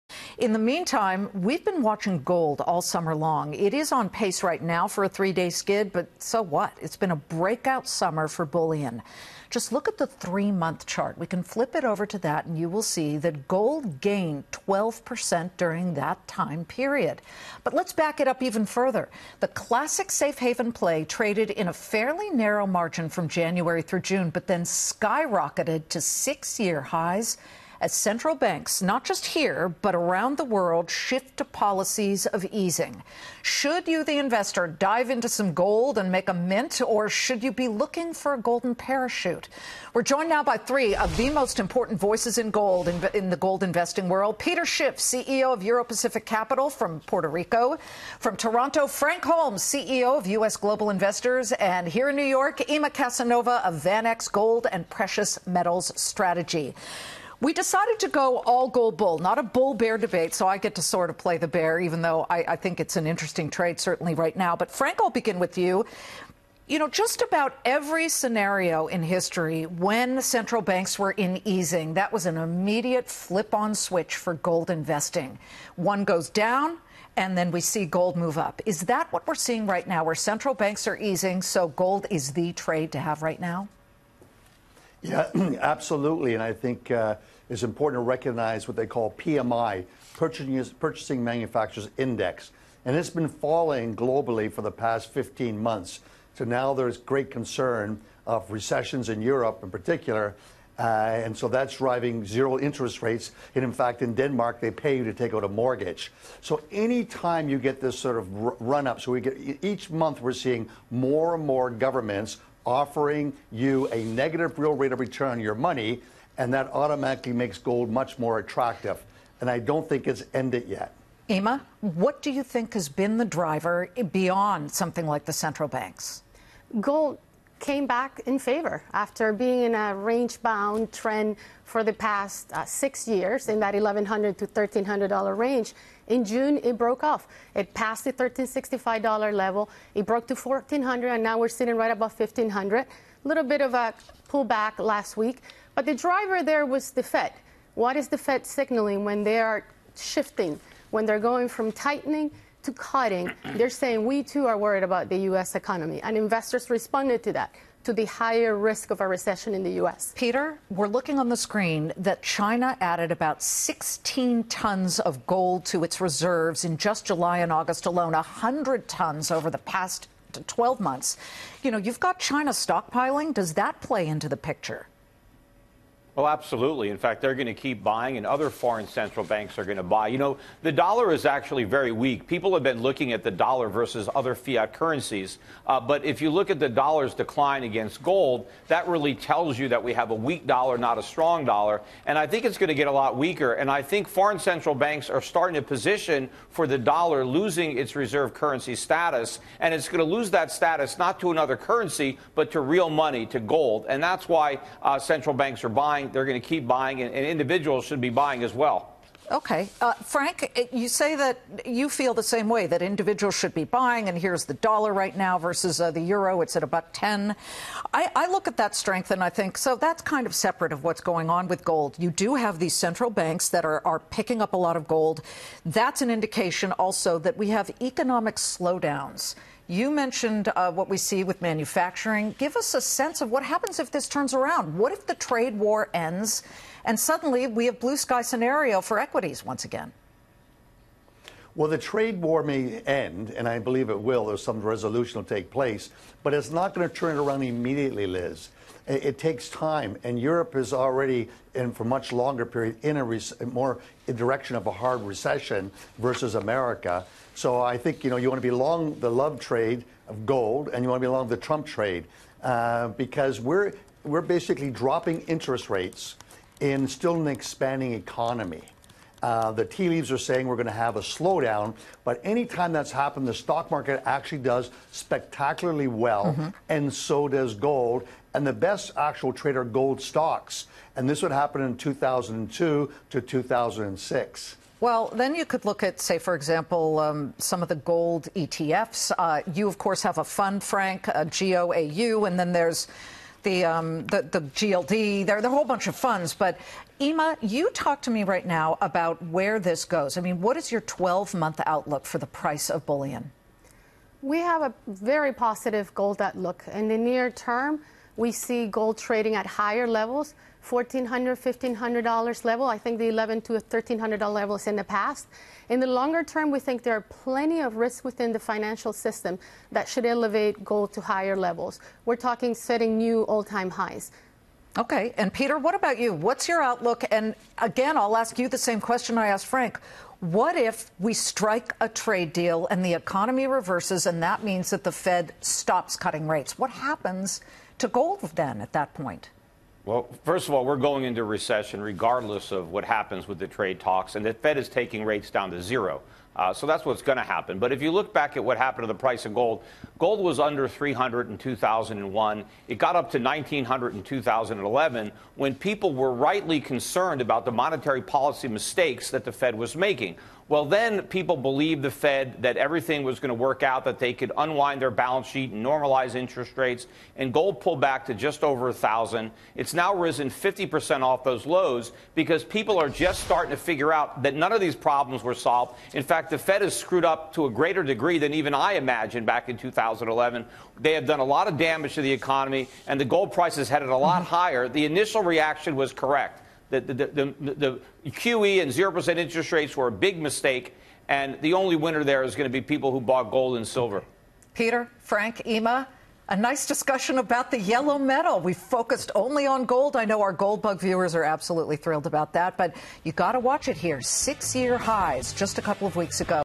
🔴Breakout Summer Bullish for Bullion
Peter Schiff on Fox Business 9/9/2019
RATE AND REVIEW this podcast on Facebook.